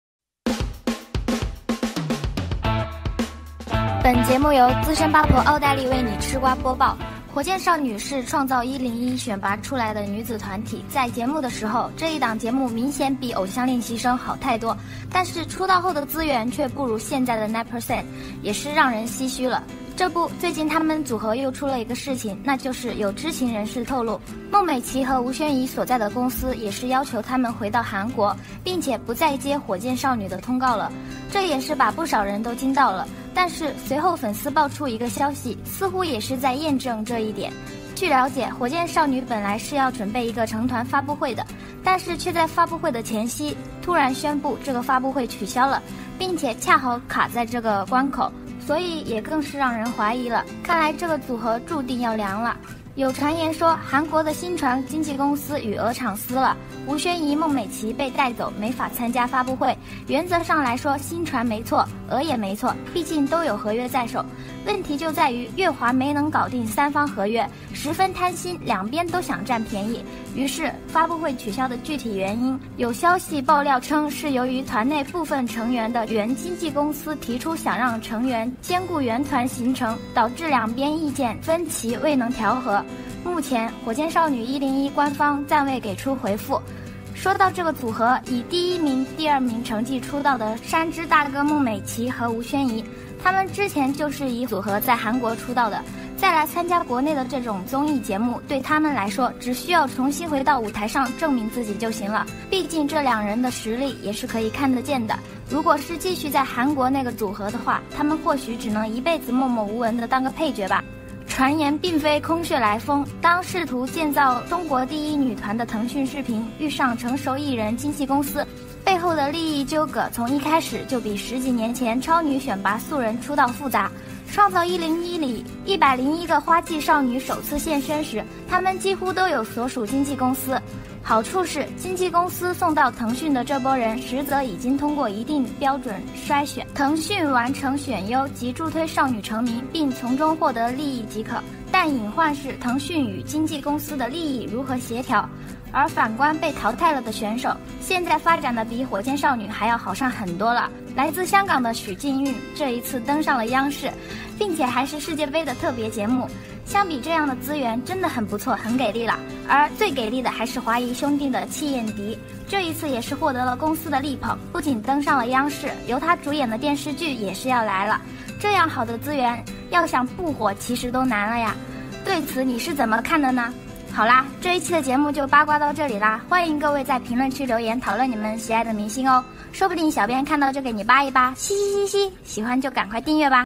火箭少女真要涼了！孟美岐吳宣儀被帶走，要用這兩人頂替她們？
孟美岐和吳宣儀所在的公司也是要求他們回到韓國，並且不再接火箭少女的通告了，這也是把不少人都驚到了，但是隨後粉絲爆出的一個消息似乎也是在驗證著這一點！